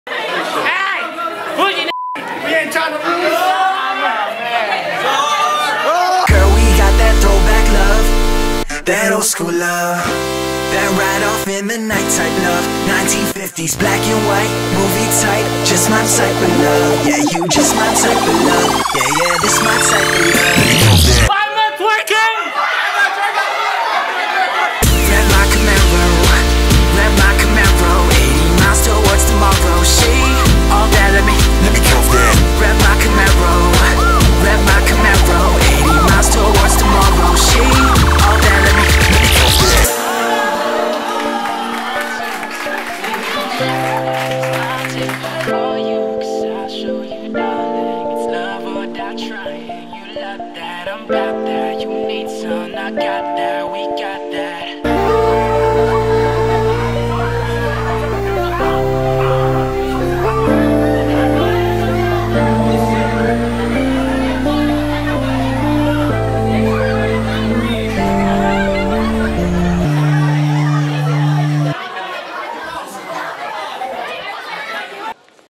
PASA's 6th General Meeting // Icebreaker
We are PASA, located at PEBW 148 on Thursdays at 7:15pm.
Hope to see you guys there!
This is an icebreaker. We have one every meeting, and each one is different. So come and enjoy the company!